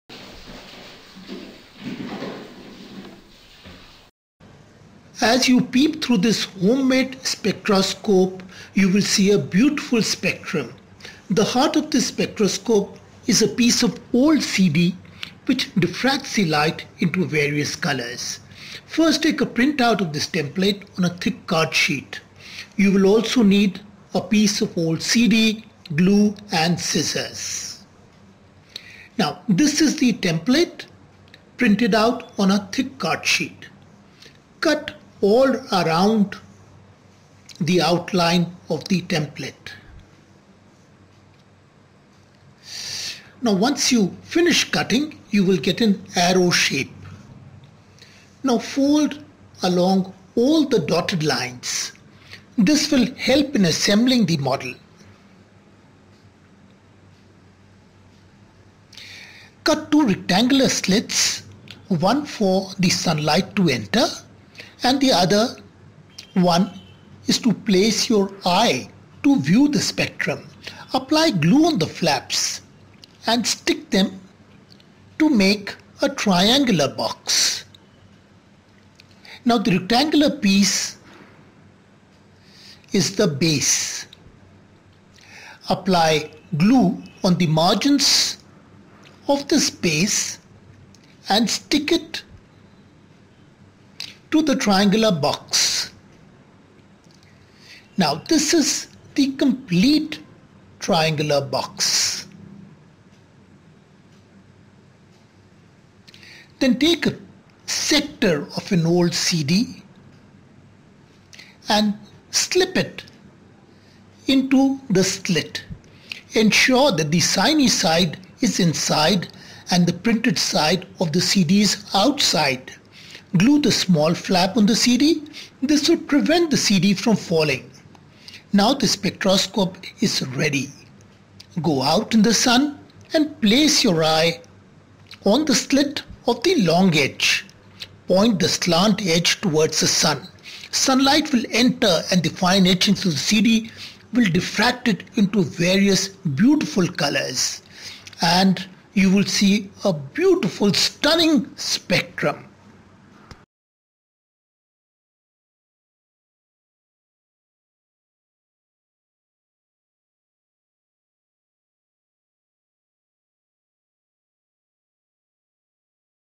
Make your own CD Spectroscope with IUCAA Scipop
Ever wondered how Astronomers find out the chemical compositions of things that mankind has never reached? They use  large Telescopes connected to Spectroscopes.

You can make this simple Spectroscope (designed by Arvind Paranjpye) and yourself use it  to look at the Spectra of various light sources and even the Fraunhofer lines in Sunlight (reflected for safety).